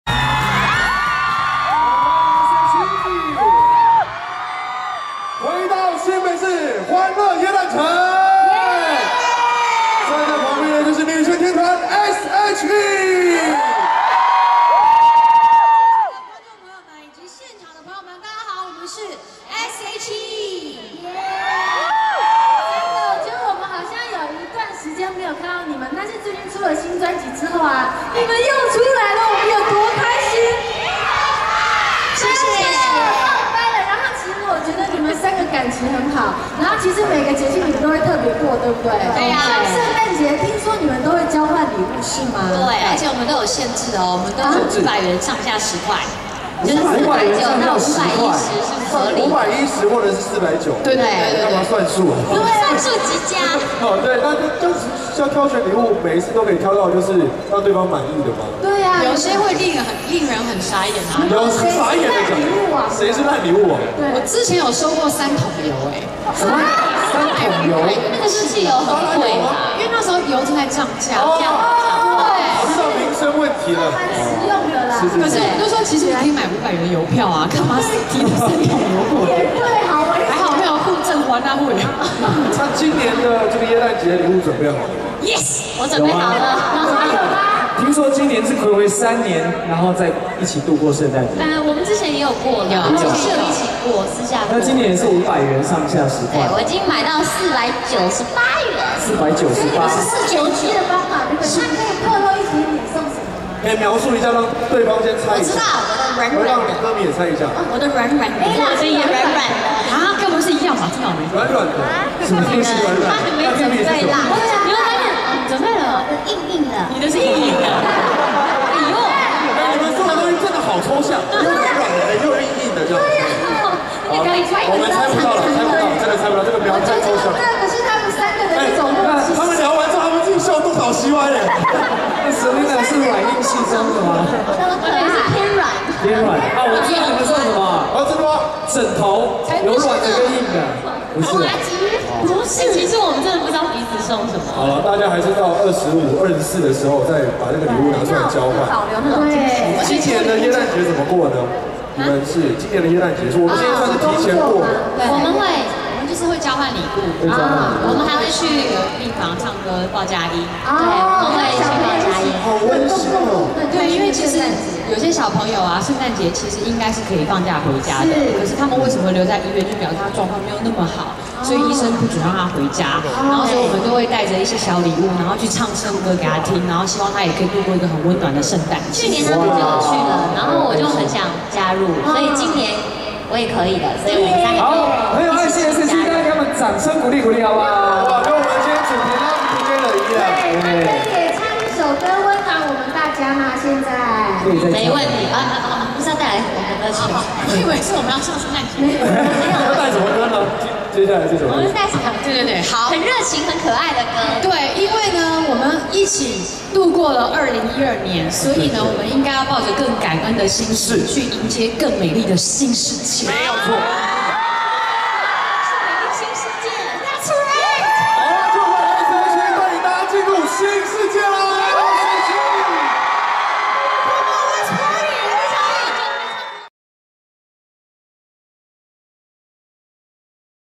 20121223 S.H.E-新北市歡樂耶誕城演唱會(3/4)訪問
use SONY XR500V